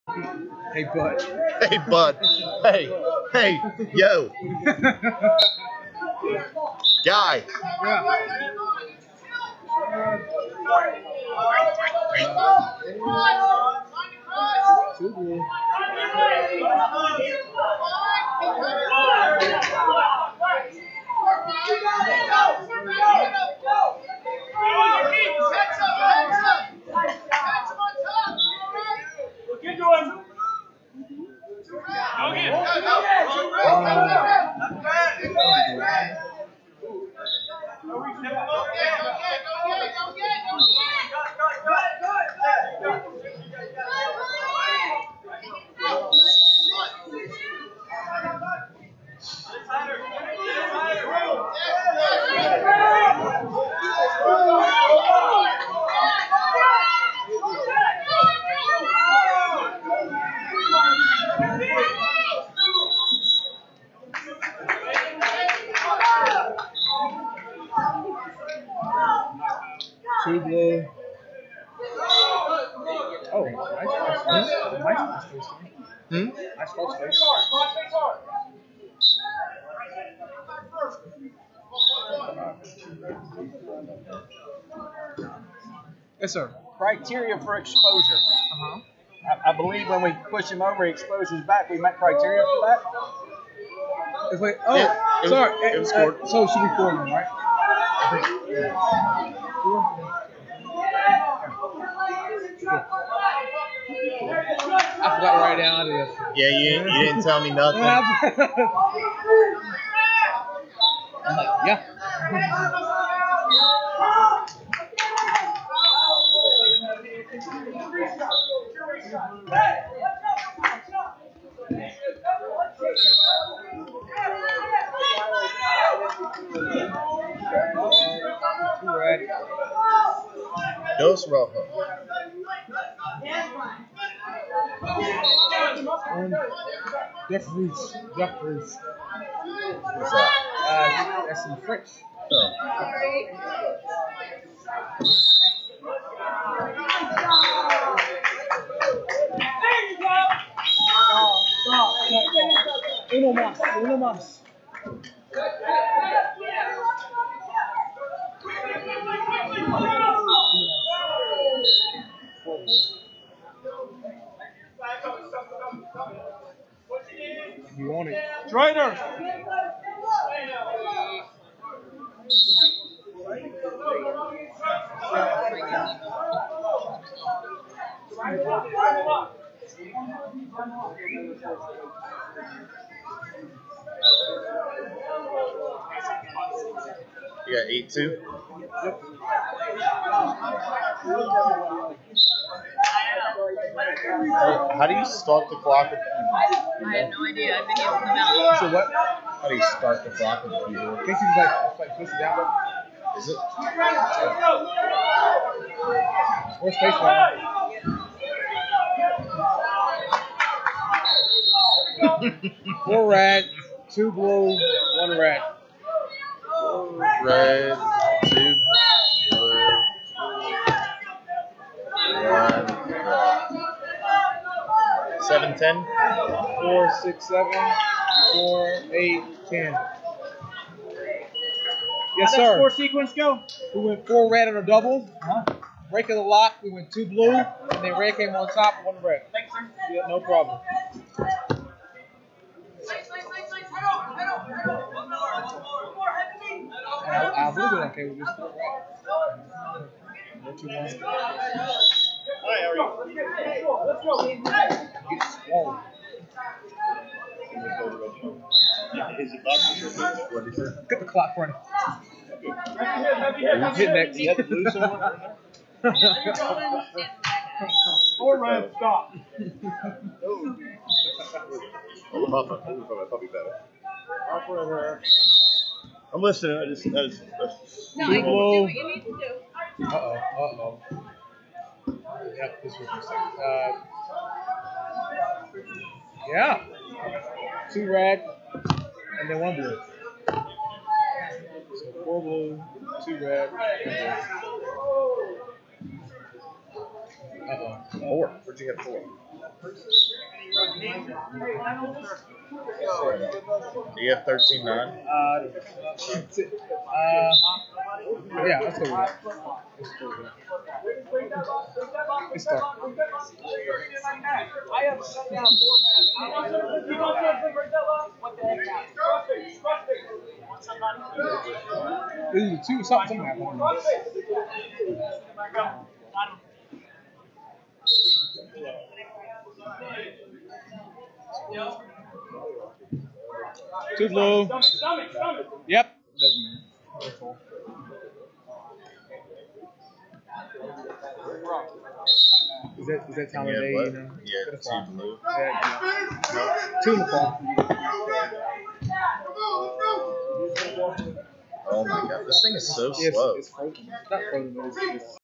Junior Men (Must Be In HS) 195 Santana Rosario South Carolina Vs Brooks Sacharczyk Tennessee
2019 Southeast Regional - Junior Men (Must Be In HS) 195 Santana Rosario South Carolina Vs Brooks Sacharczyk Tennessee